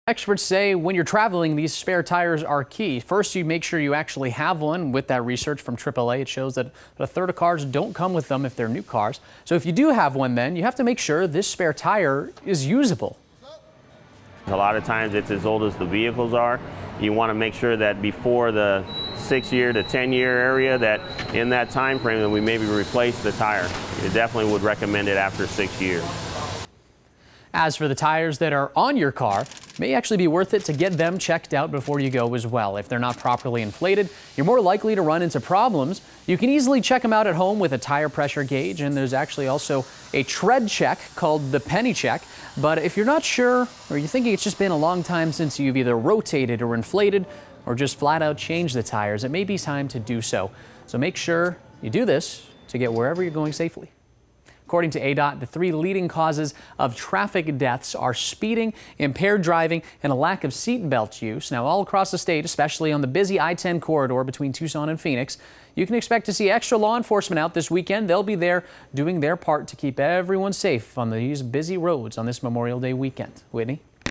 Hitting the road for Memorial Day? Make sure you have a spare tire
As thousands of Arizonans will hit the highways this Memorial Day weekend, law enforcement officials and car mechanics want to make sure everyone gets to their destinations safely.